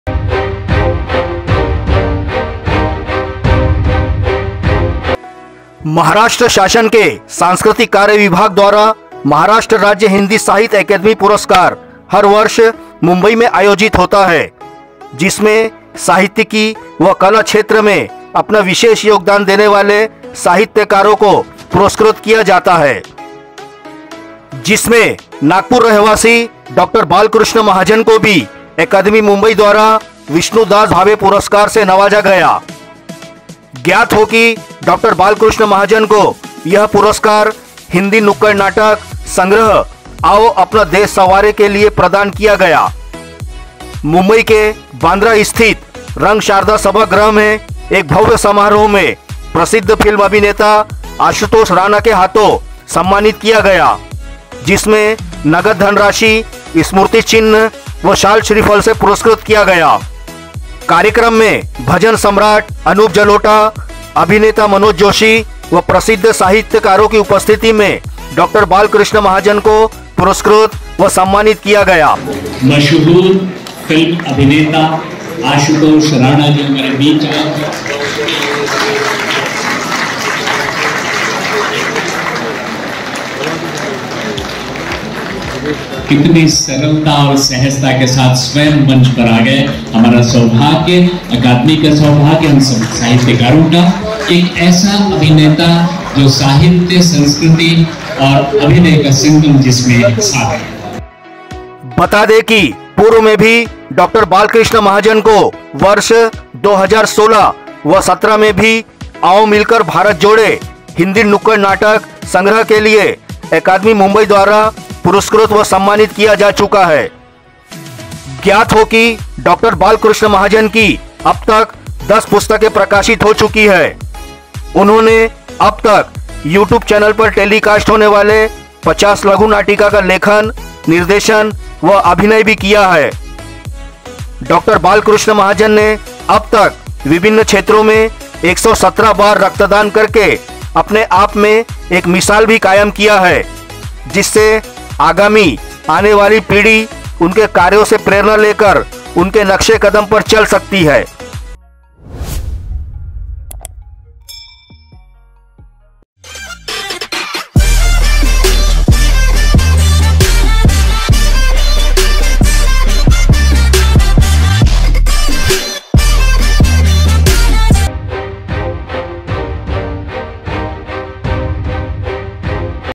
|| डॉ. बालकृष्ण महाजन हुए पुरुस्कृत || हिंदी साहित्य अकादमी पुरस्कार ||
मुंबई ll महाराष्ट्र राज्य हिंदी साहित्य अकादमी  || डॉ. बालकृष्ण महाजन हुए पुरुस्कृत ||